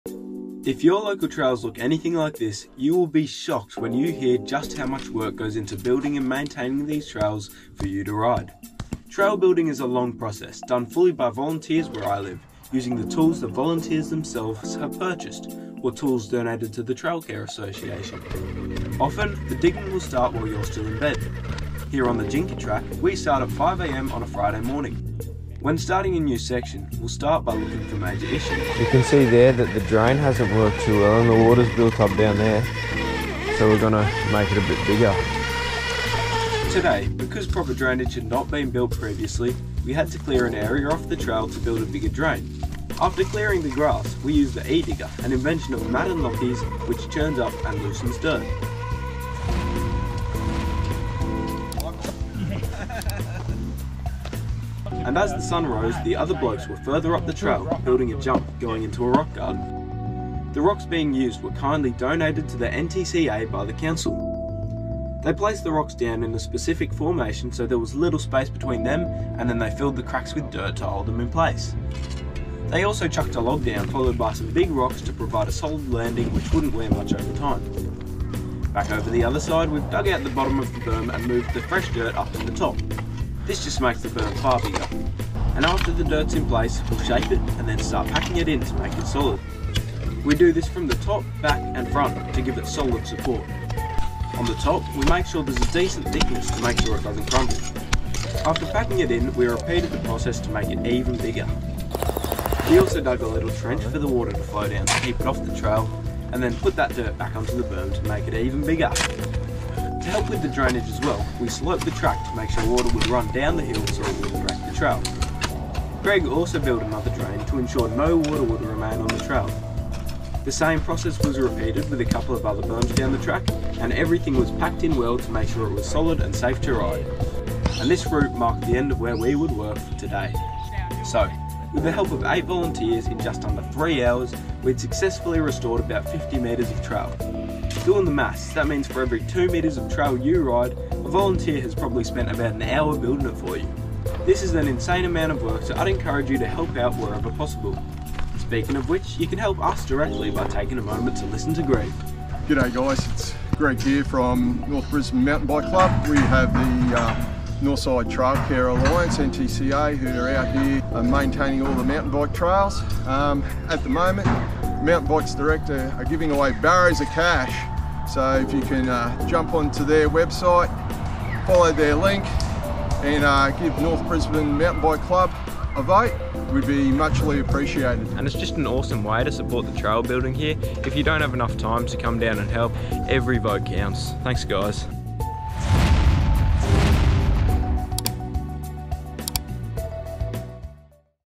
The TRUTH Behind Mountain Bike Trails
Have you ever wondered how much work goes into the trails you ride? Today, I'll be taking you through the process of building a trail, here on the Jinker Track.

Heaps of volunteers put in loads of effort, so I'd encourage you to head down to your locals and give a hand with maintenance.

Also, if you could vote for North Brisbane Mountain Bike Club, that would be much appreciated!